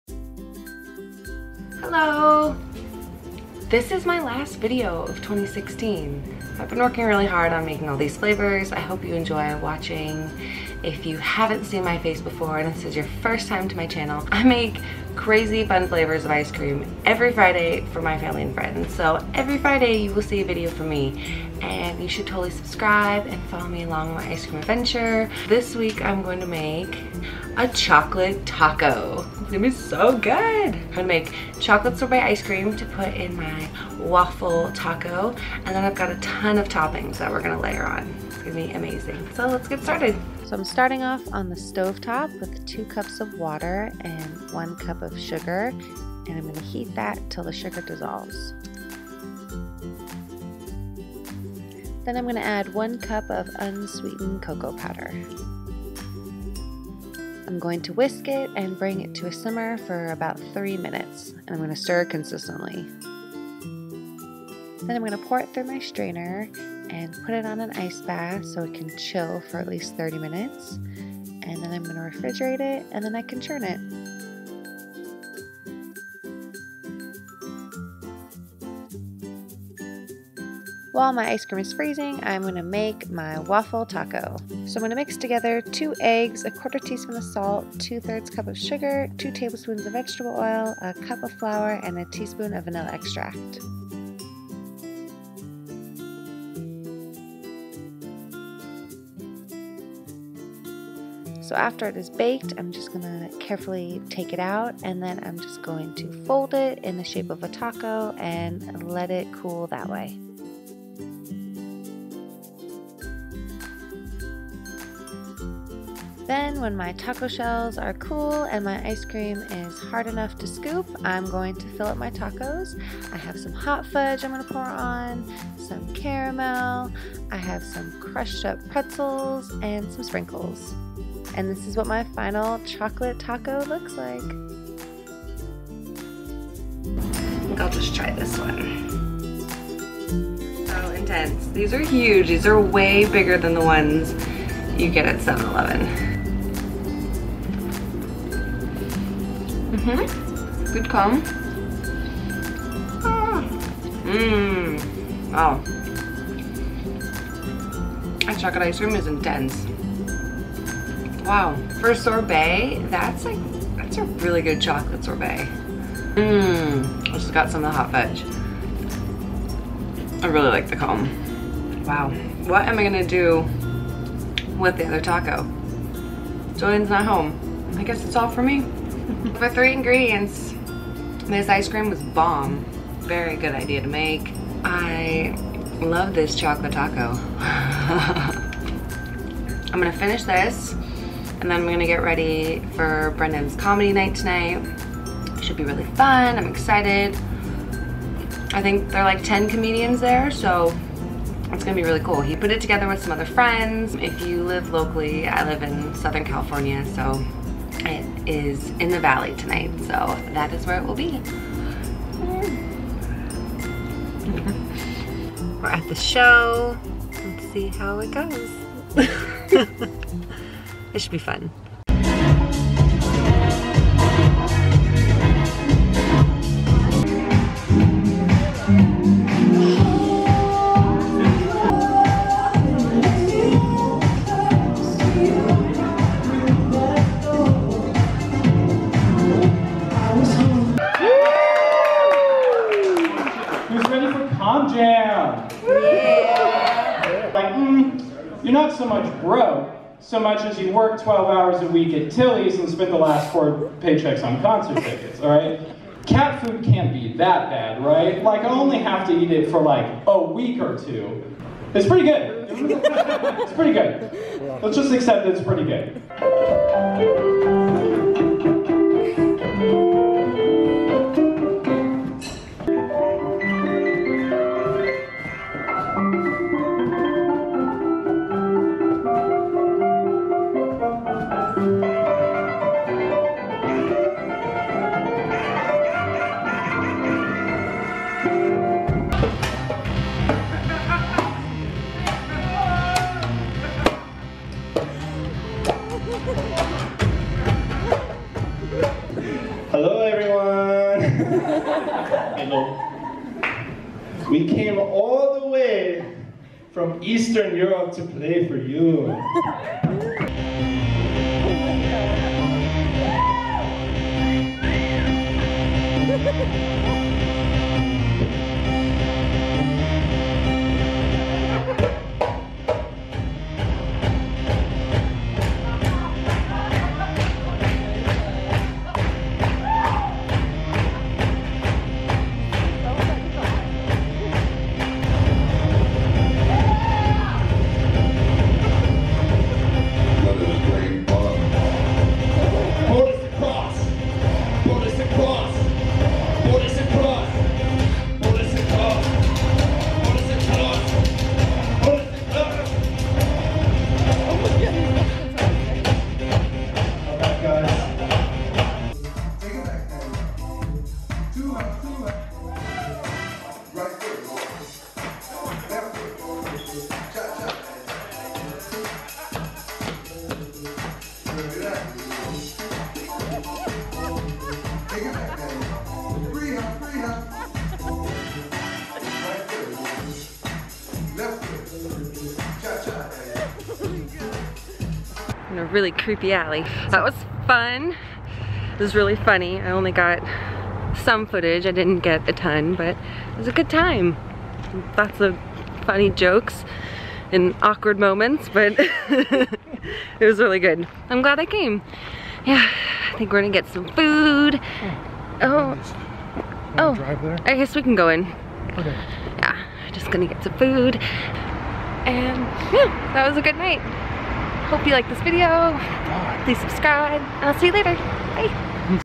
DIY ULTIMATE CHOCOLATE TACO || THE IceCreamQueen
Hey! This week for my last video of 2106 I wanted to make something crazy. So I made my own version of a chocolate taco. It was incredible. I hope you enjoy!

Hi! I'm Amanda and I love making fun & unique ice cream flavors for my friends & family.